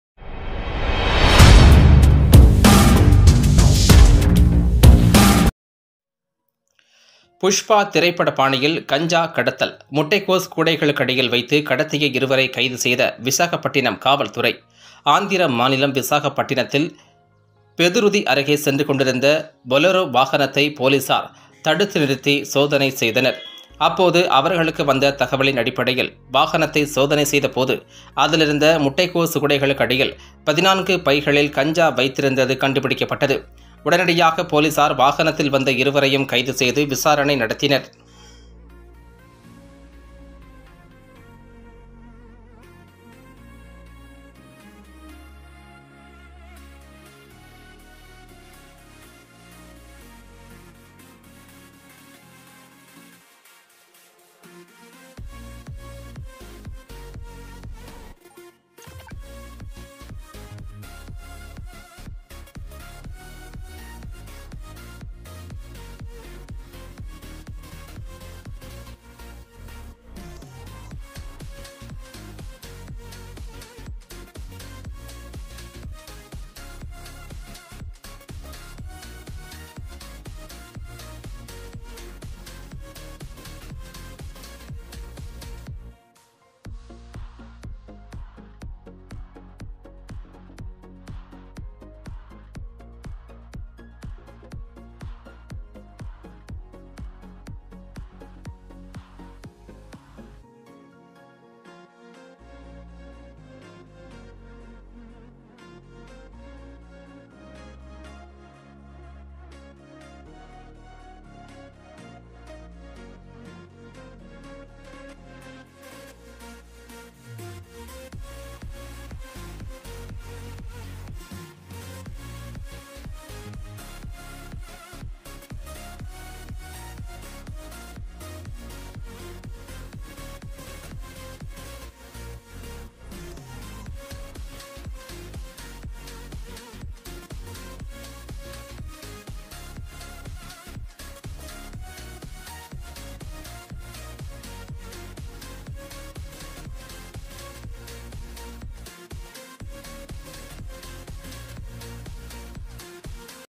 புஷ்பா திரைப்பட பாணியில் கஞ்சாவை முட்டைகோஸில் வைத்து கடத்தியவர்கள் கைது
Andhra Visakhapatnam kanja issue news#